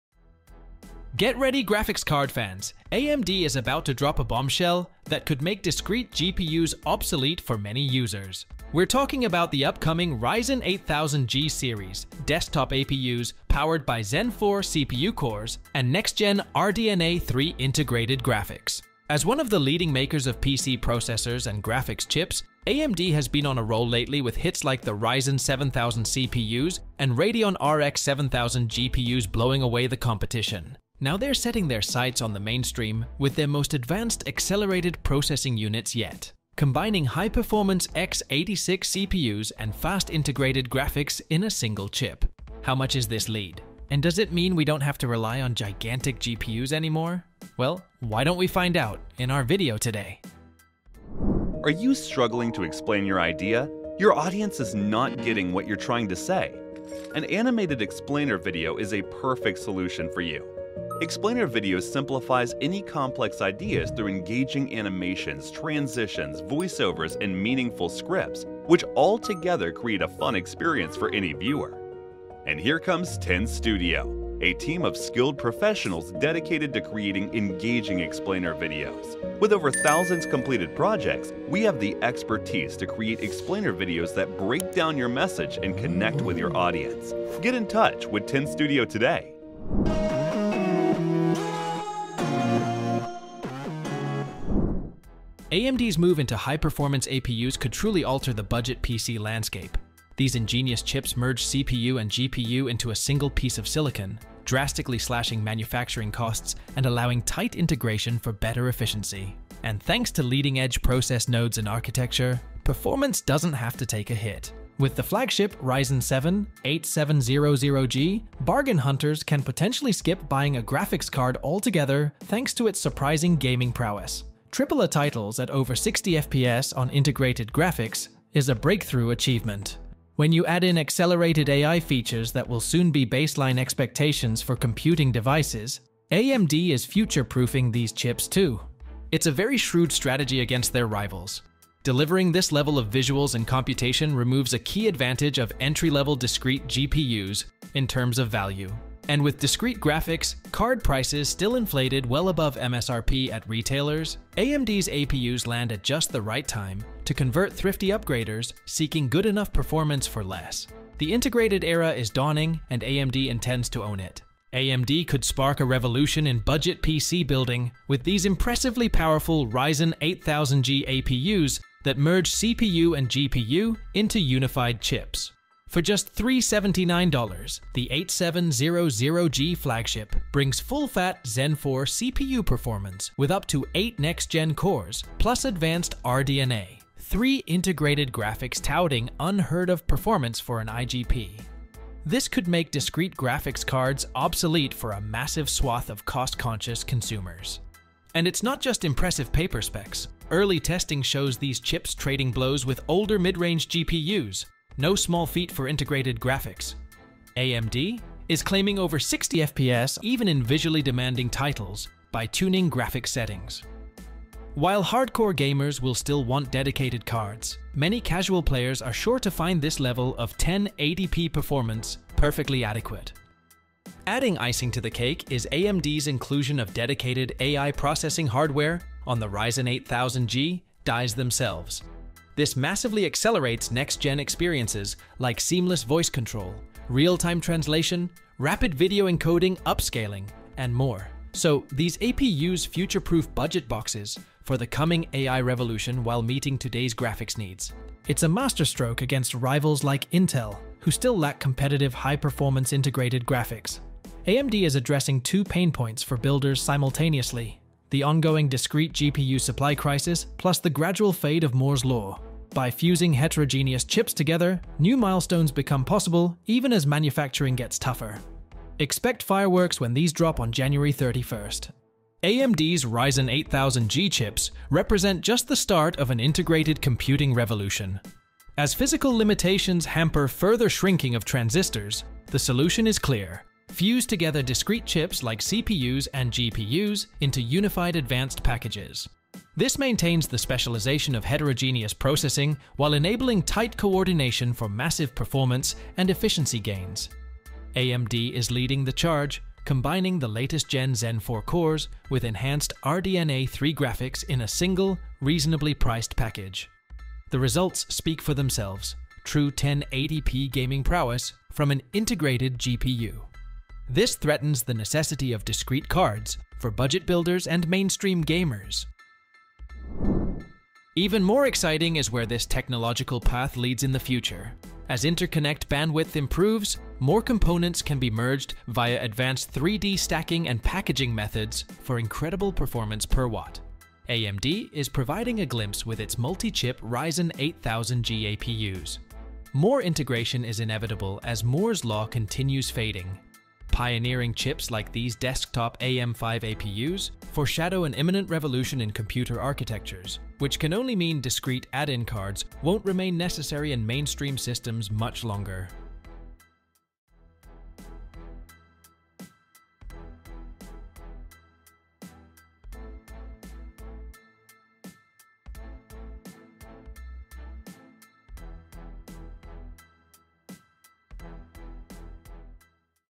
Upcoming AMD Ryzen 8000G Chips | Say Goodbye to GPUs
AMD stuns budget builders with revolutionary Ryzen 8000G desktop chips fusing Zen 4 CPUs and RDNA3 graphics, delivering true gaming prowess without discrete GPUs. Could these surprisingly capable "fusion" chips make standalone graphics cards obsolete for mainstream PC needs?